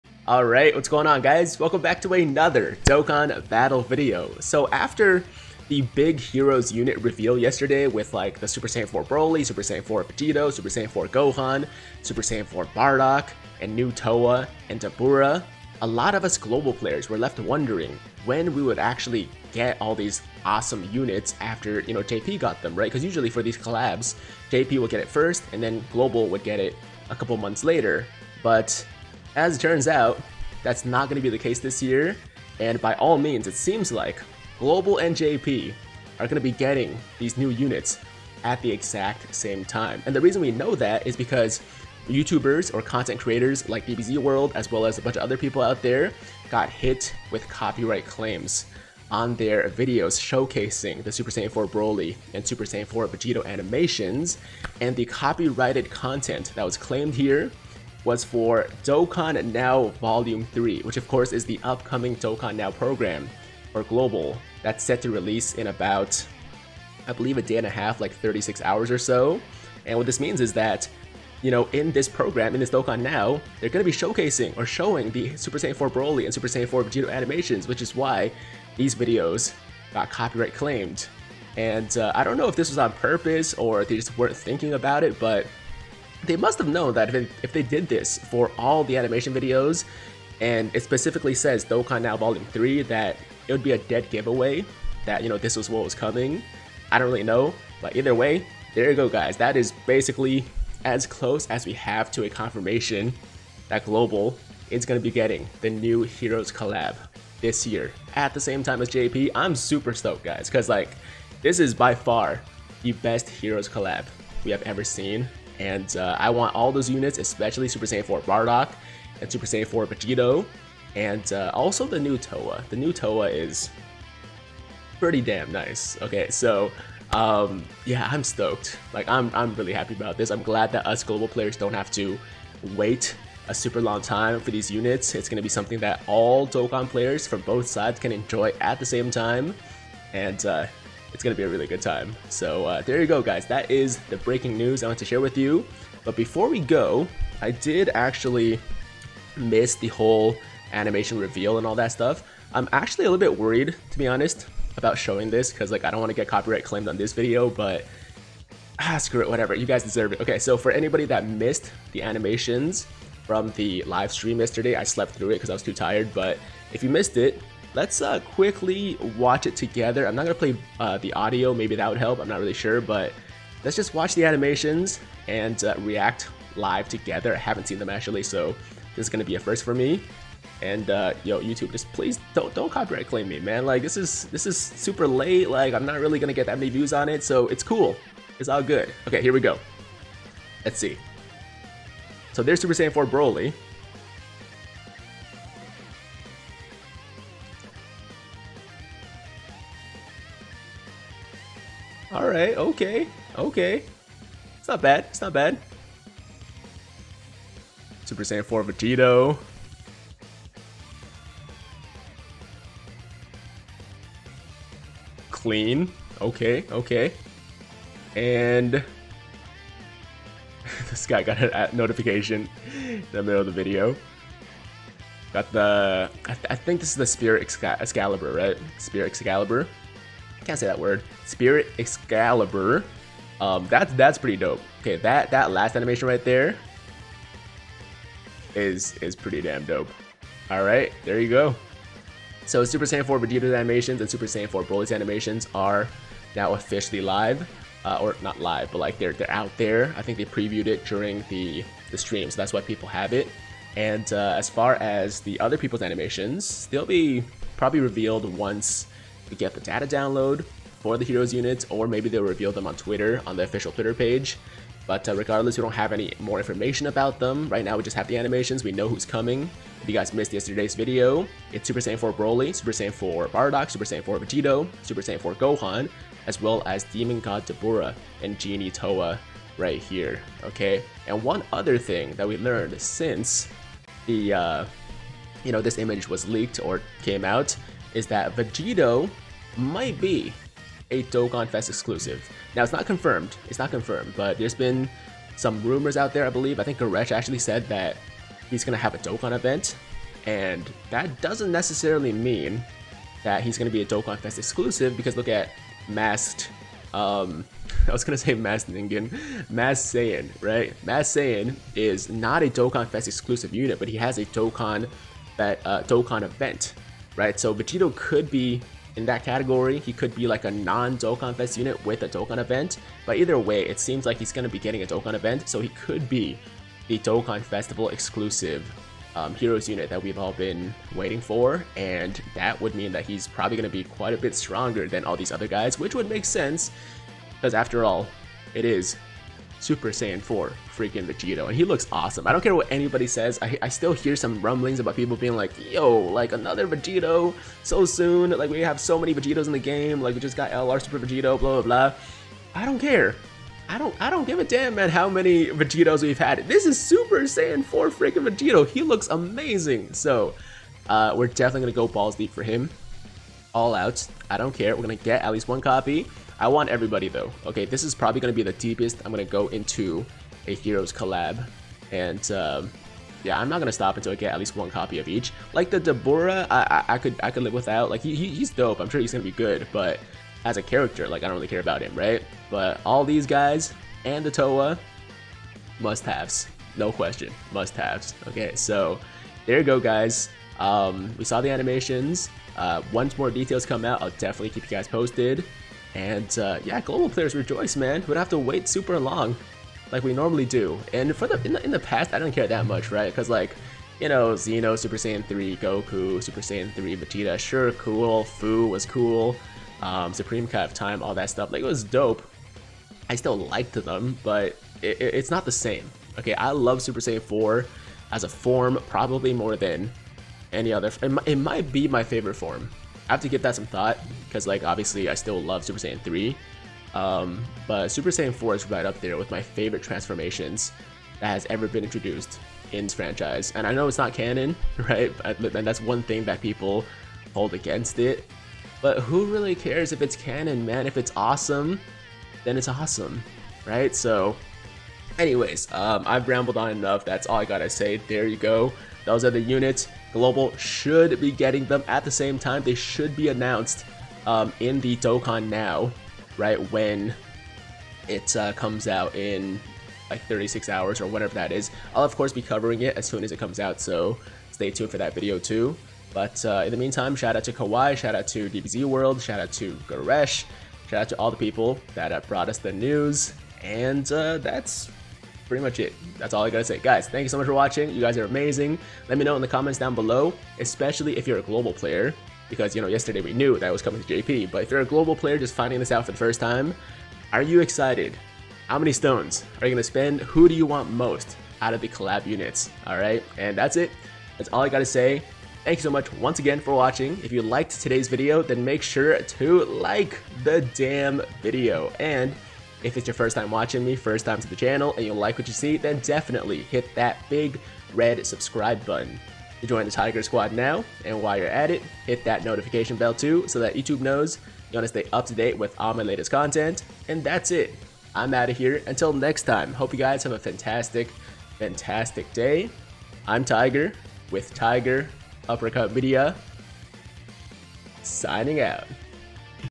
GLOBAL & JP JOINT HEROES CAMPAIGN CONFIRMED?! + SSJ4 Vegito & SSJ4 Broly Animation REACTIONS
GLOBAL PLAYERS REJOICE! We won't have to wait for these AMAZING characters!